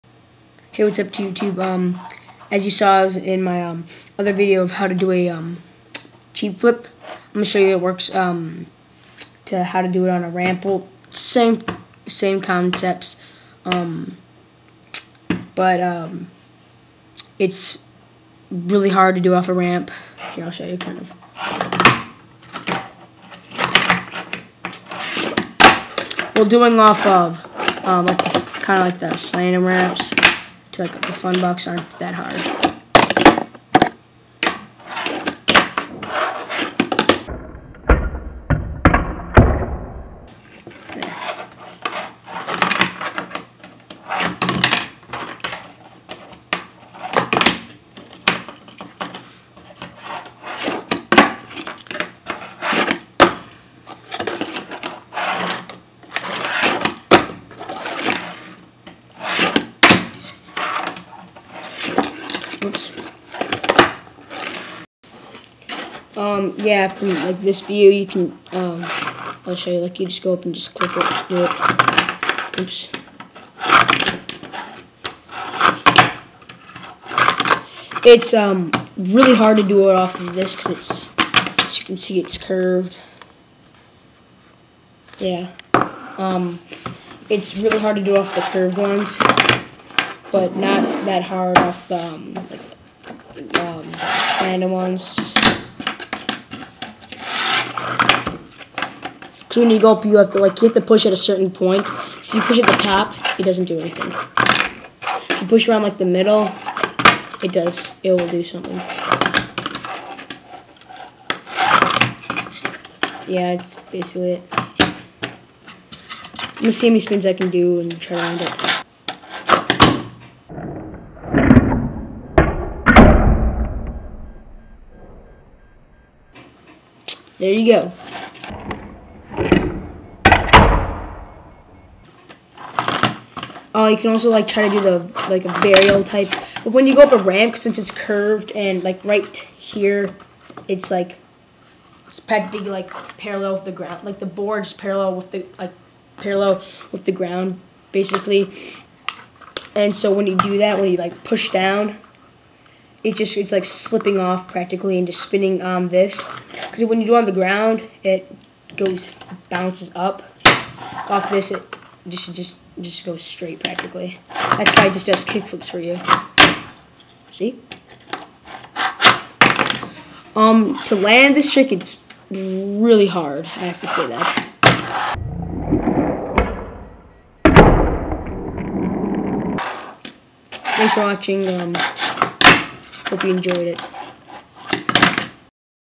tech deck cheap flip 2
how to do a cheap flip on a ramp. its really hard to land and to spin the board.